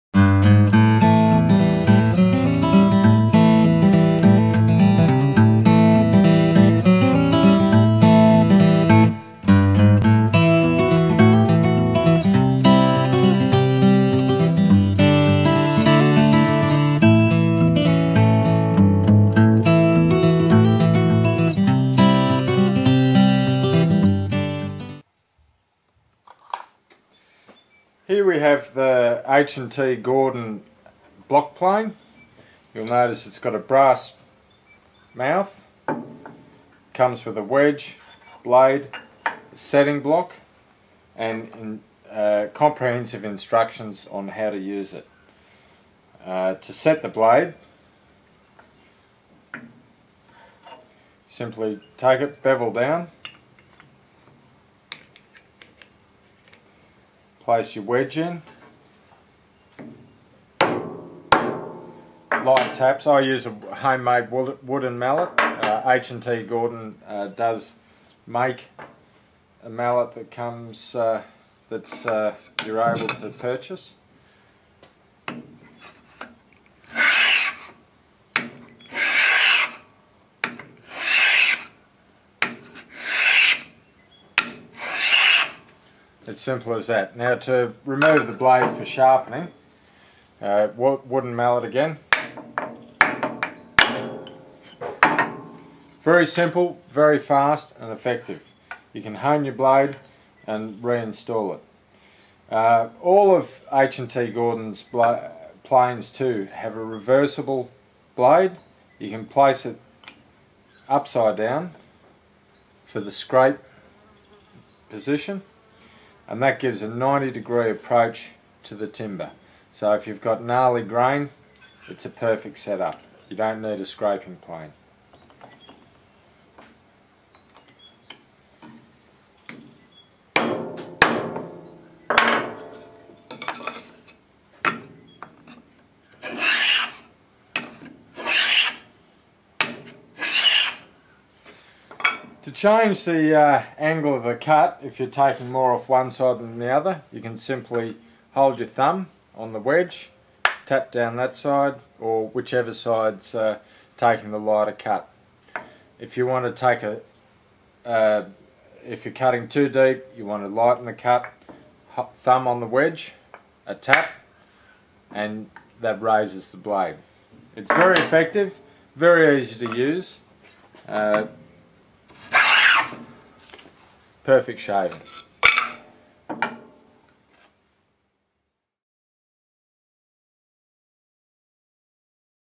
Australian HNT Gordon Block Plane
Setting up an Australian HTN Gordon Block Plane